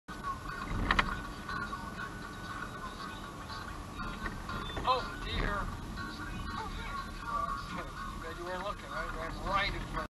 Near Collision With Deer Main Siding Rd Caribou Maine 11 05 17
Driving cautiously paid off